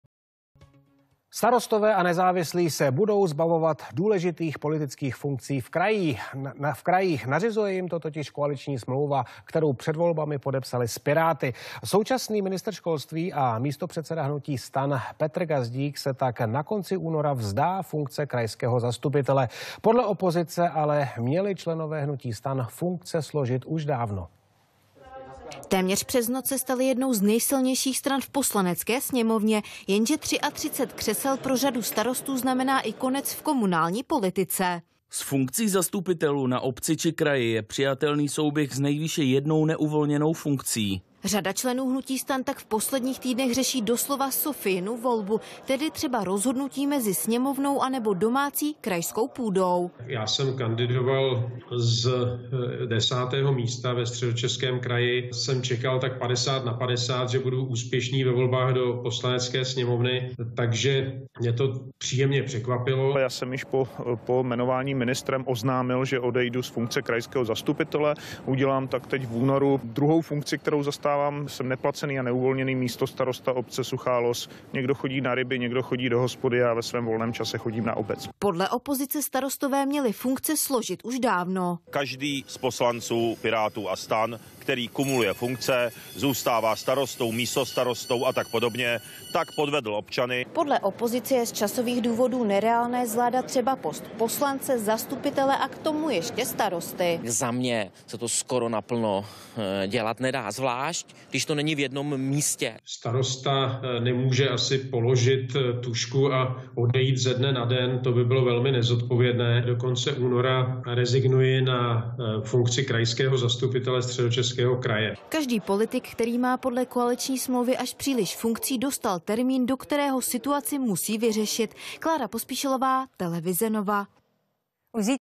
TV Nova - STAN a kumulace funkcí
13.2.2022
Co v reportáži nezaznělo - hlavně jsem zdůraznil, že je to vnitřní věcí STANu a je pak na jejich voličích, jak se jim to bude zamlouvat. Teprve poté jsem mluvil z vlastní zkušenosti o tom, že se to stihnout fakticky na 100% výkon nedá (vzdal jsem se pozice předsedy zastupitelského klubu i přesto, že mám obojí v Praze přes most).